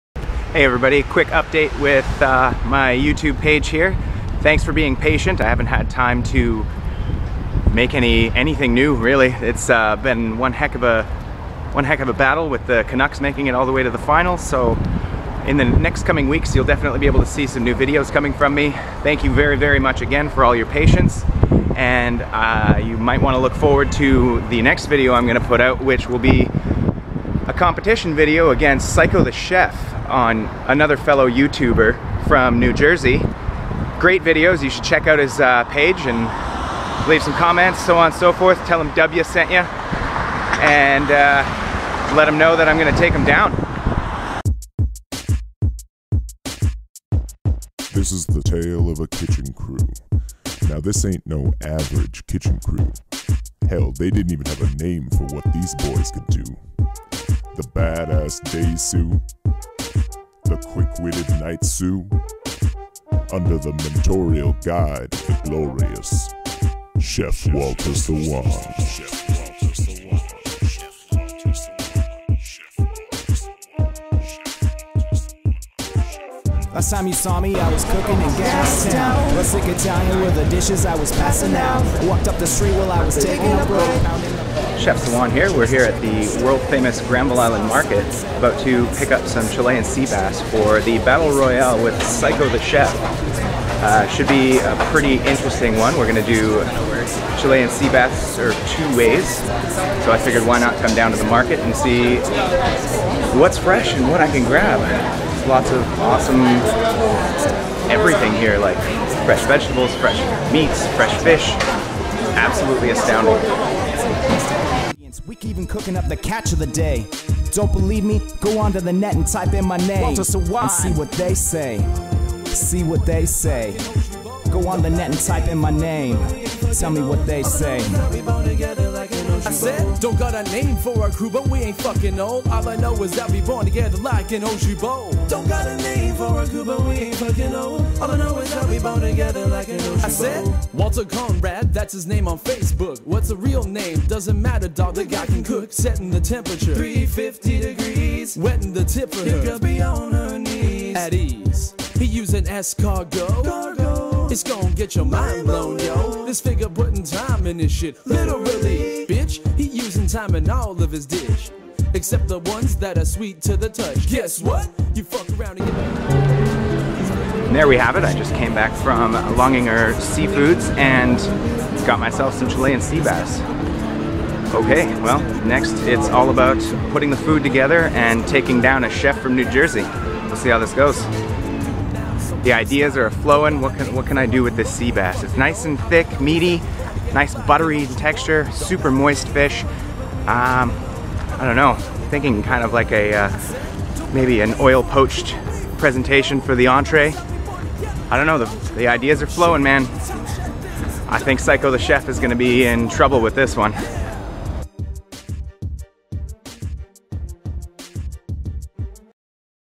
Trip To Granville Island Public Market (video update 2)
Hit up Granville Island Public Market. went to Longliner Seafoods to pick up some Patagonian Toothfish. brought here to Canada from a regulated Fishery.

The Shaky footage is due to the random Godzilla Attacks!

The Track used in the background, is a look into the future of music!